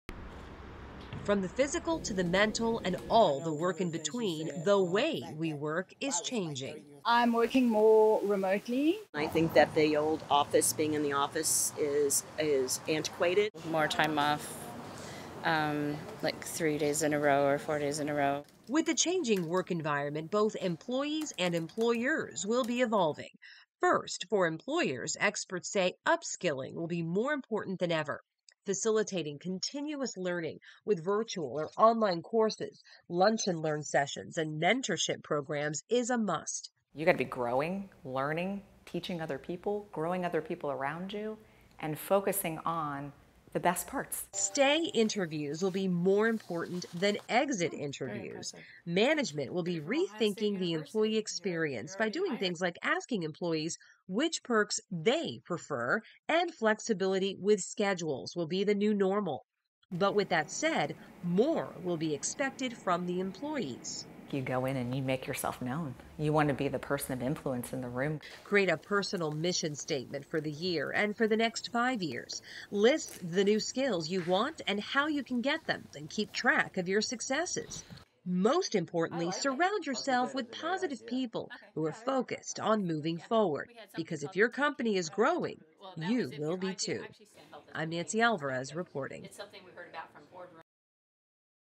NEW YEAR, NEW WORKPLACE: HOW JOBS WILL EVOLVE IN 2023
There’s no story evolution like the one between you and your boss. The new year brings on a new workplace dynamic for everyone. The new year will demand more work from you, and your employer